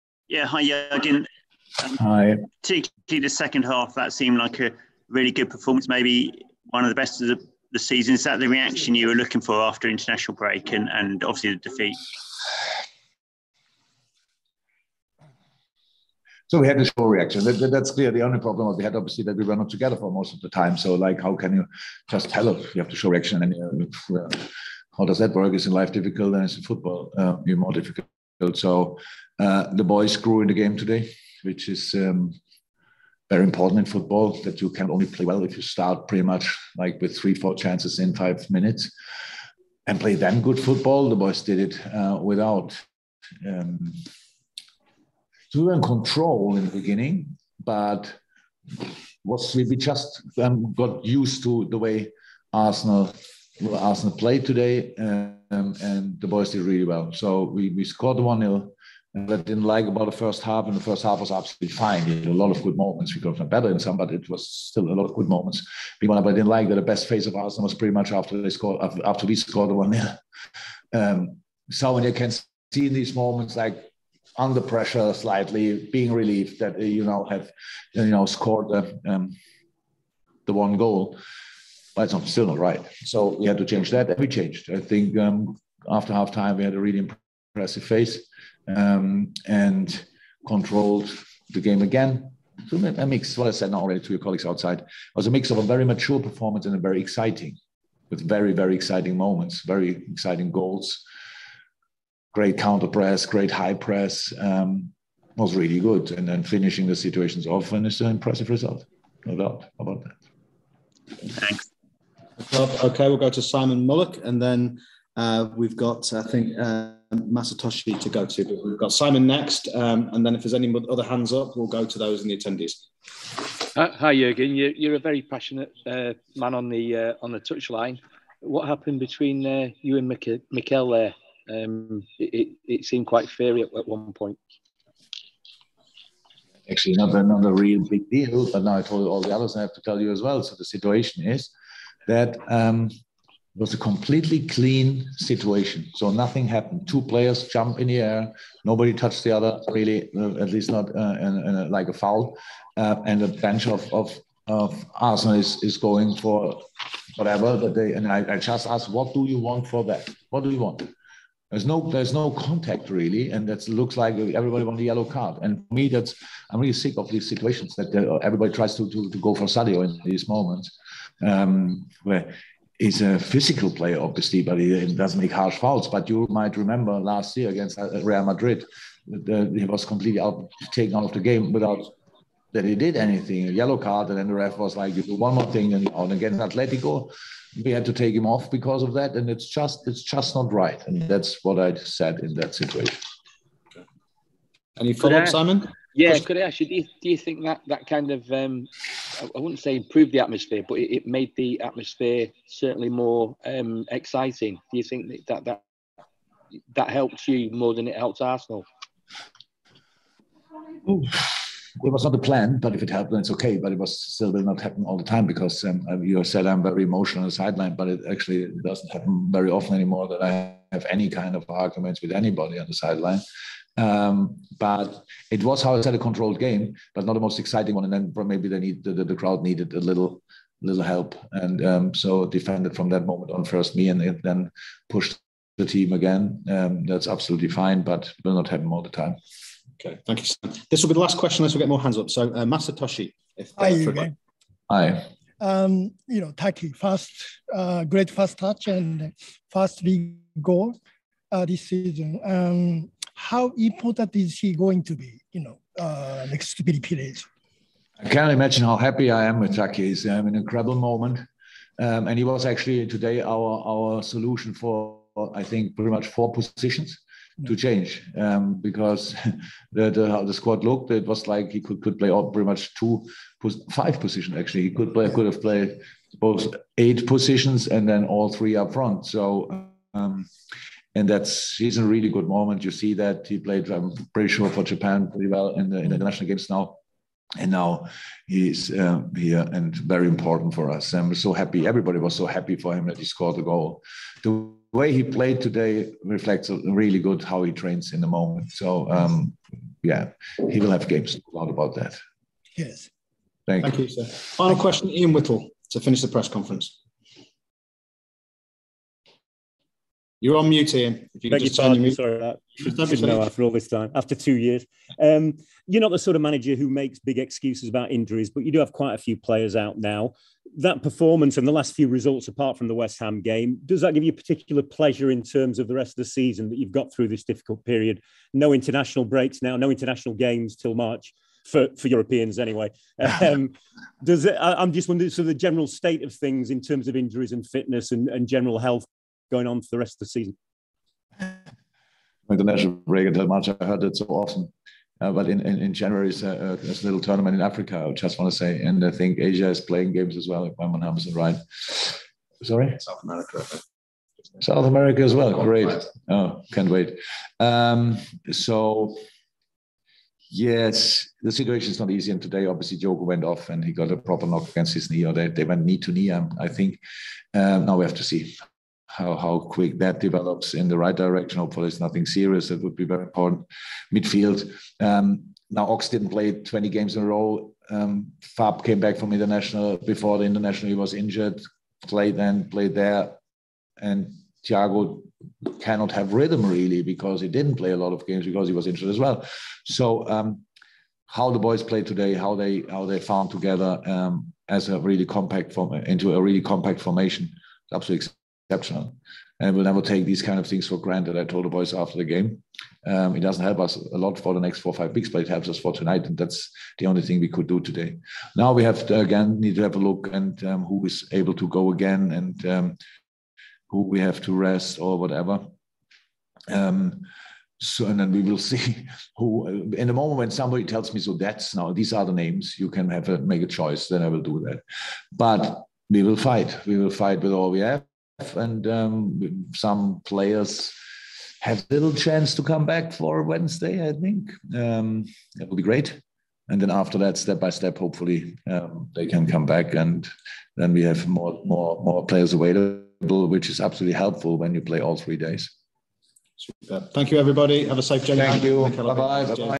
‘Absolutely Exceptional’ | Jurgen Klopp Press Conference | Liverpool 4-0 Arsenal
Jurgen Klopp gave a glowing report of this Liverpool side after they dispatched Arsenal 4-0 at Anfield in the Premier League.

The Reds boss was asked about his heated side-line debate with Mikel Arteta after the visitor's bench erupted following a strong challenge from Sadio Mane.

00:00 - Jurgen Klopp’s verdict on Liverpool 4-0 Arsenal
02:03 - ‘No Big Deal’ - Jurgen on his heated argument with Mikel Arteta
03:25 - ‘I’m very emotional’ - Klopp on his sideline battle with Mikel
04:10 - ‘Happy with Taki’ - Jurgen Klopp on the performance of Takumi Minamino
05:08 - Jurgen Klopp asesses Liverpool’s remaining injuries ahead of the December run

JOIN us on 24th November in Dublin for Blood Red LIVE tickets available: bloodred.ie